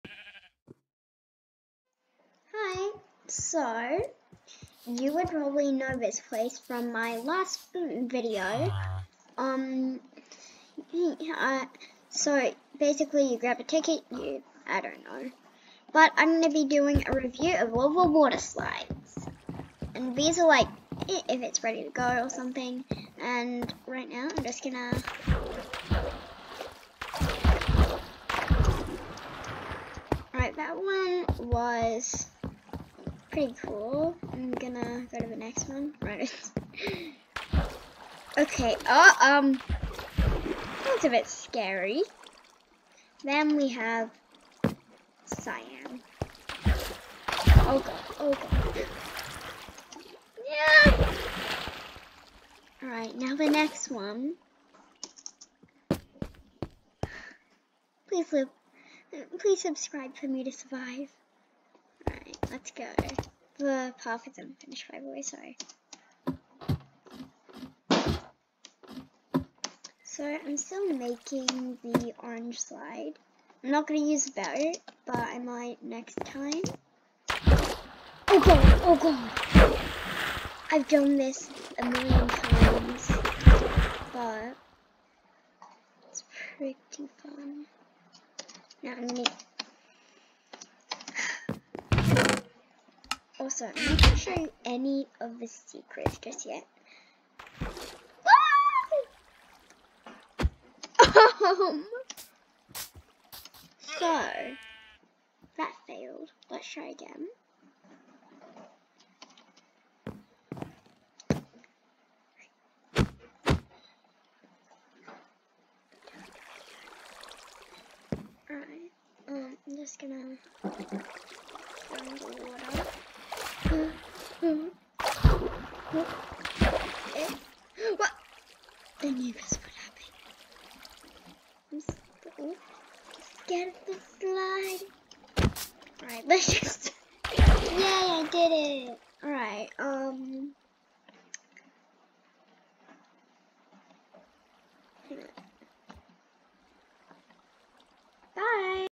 Slides
I am so scared after that red slide hehe.